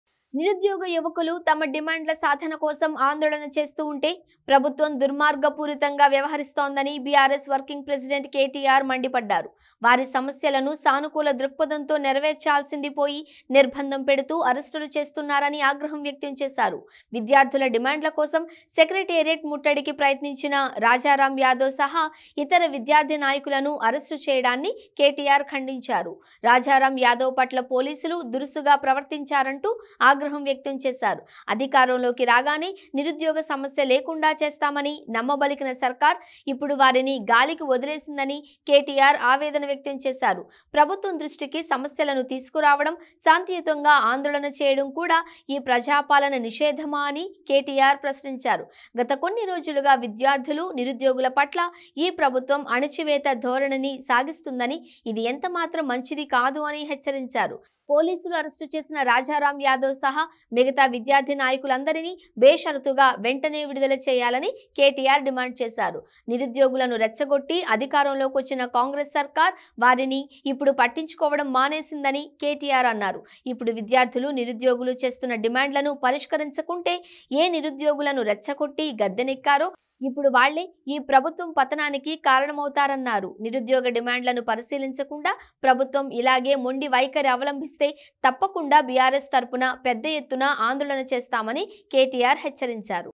కాంగ్రెస్ పార్టీ దౌర్జన్యాలు .. | KTR condemned the arrest of student leaders | CM Revanth Reddy
కాంగ్రెస్ పార్టీ దౌర్జన్యాలు .. | KTR condemned the arrest of student leaders | CM Revanth Reddy | Aadya TV